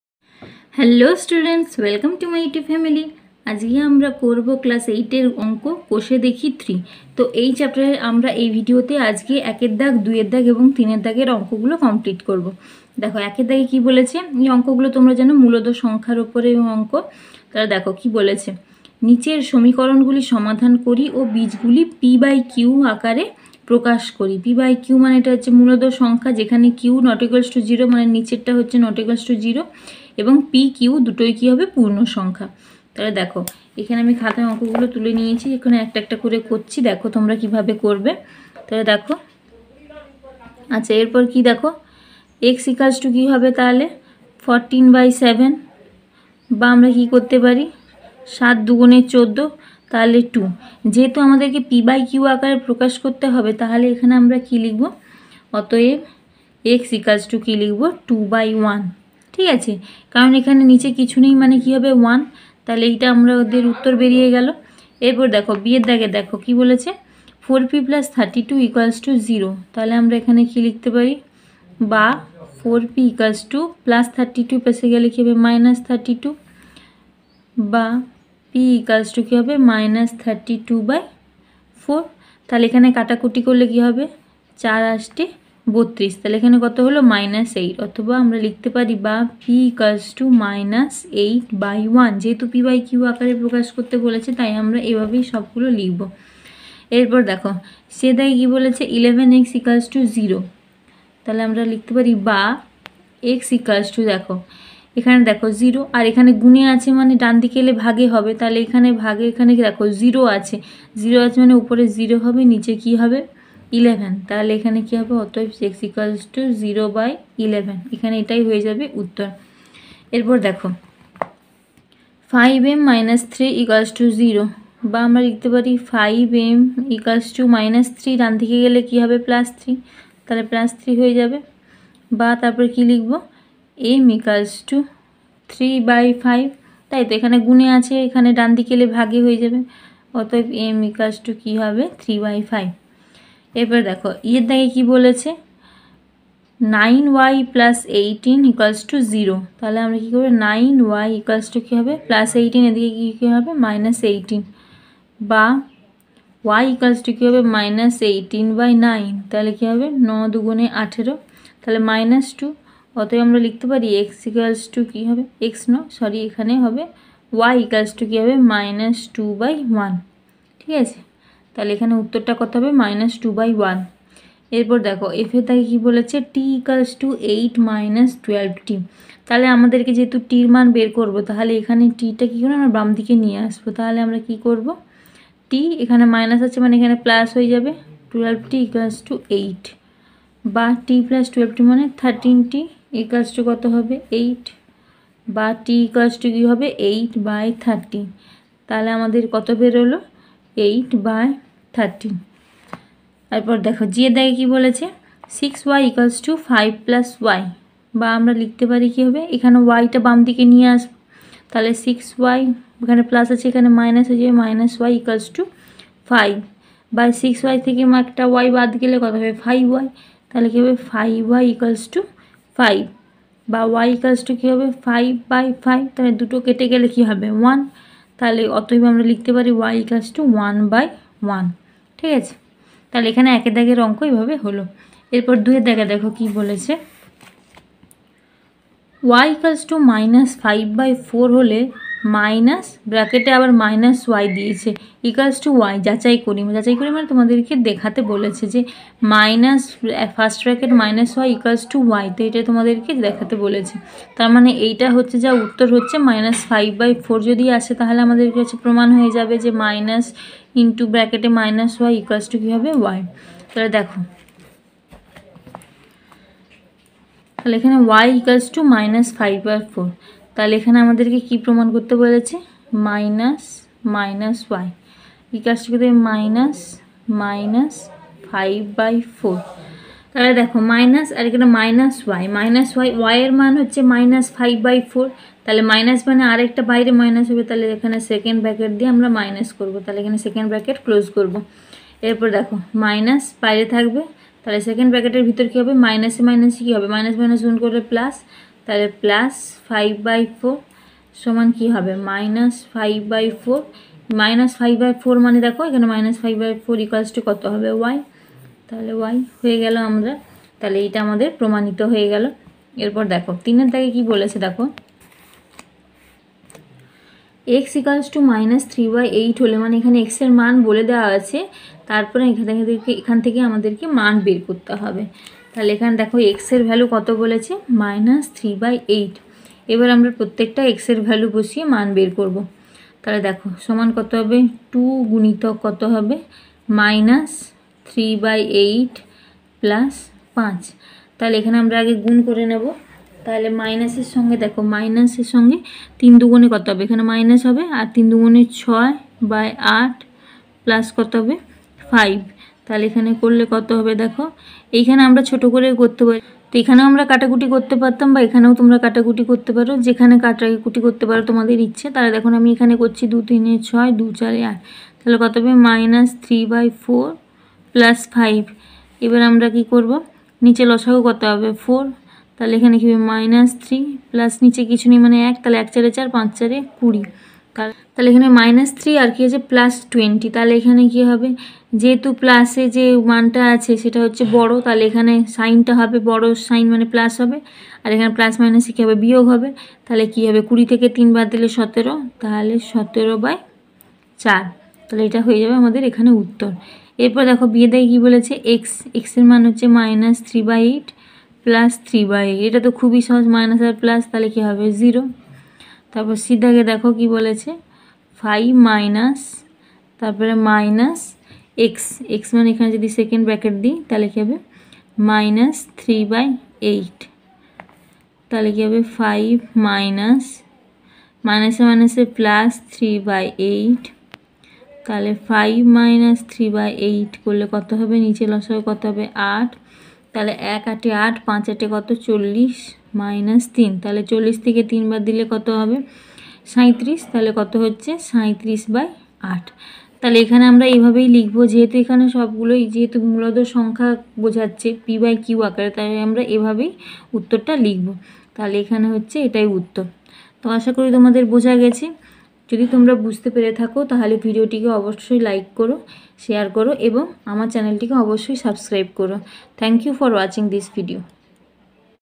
KOSHE DEKHI 3,QUESTION NO.S 1,2&3,MATH,CLASS 8,W.B.B.S.E,
In this video I have solved question no.s 1,2&3 of the chapter Koshe dekhi 3 from class 8.